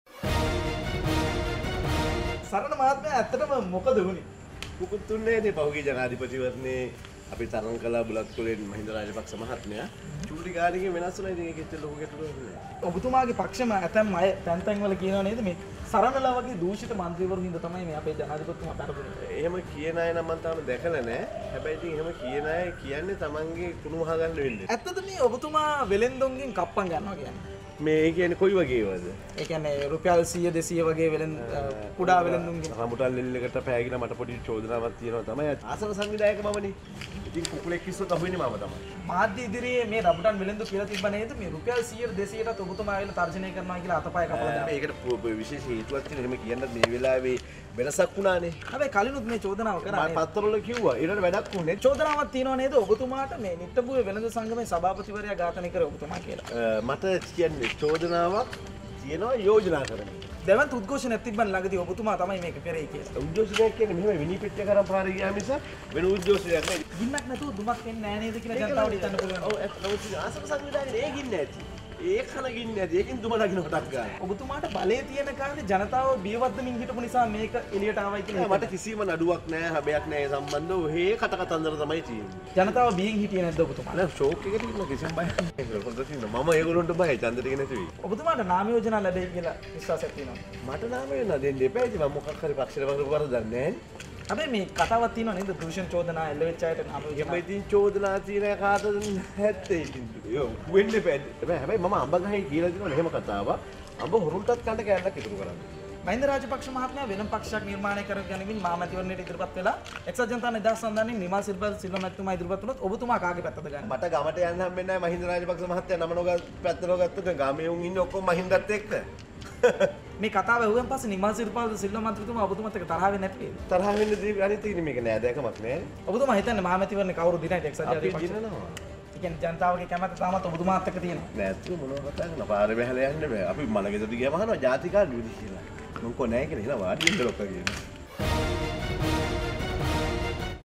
What Happened? Sarana Gunawardena
Former Deputy Minister Sarana Gunawardena reveals ‘What Happened?’, who he will support as Prime Ministerial candidate, which party will win General Election and responds to the various allegations leveled against him.

අද මොකද වුණේ කතා බහ සරණ ගුණවර්ධන මන්ත්‍රීවරයා සමග.